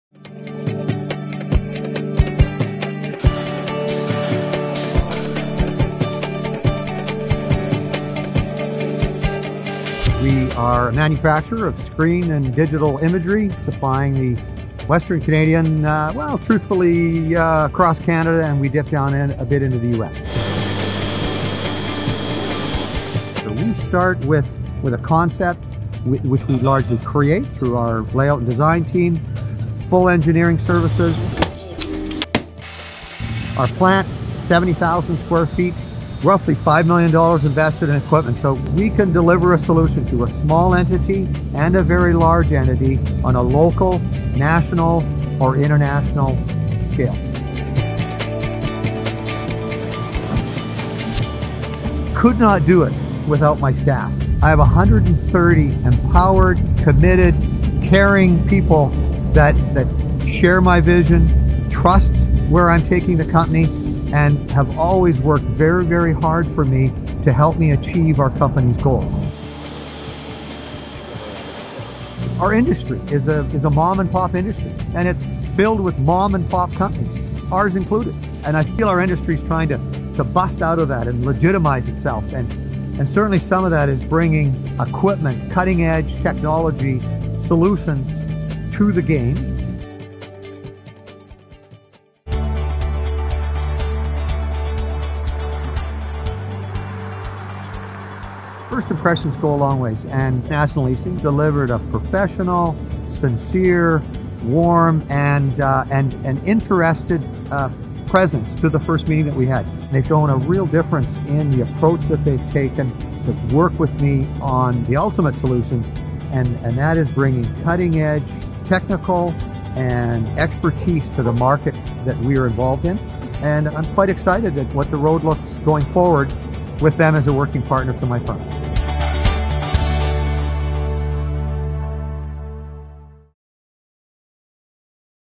Equipment Solutions for the Print Industry | National Leasing & Cowan Graphics
| Hear President Blain MacMillan's take on today's graphics industry and what it takes to get to the top. From shops in Edmonton, Calgary and Saskatoon, Cowan Imaging Group is breaking the "mom and pop" mold by providing design, layout, graphics, printing and more to international customers. With $5 million invested in equipment they bring cutting edge solutions to customers. He'll tell you why it takes a trusted financial advisor to make this possible.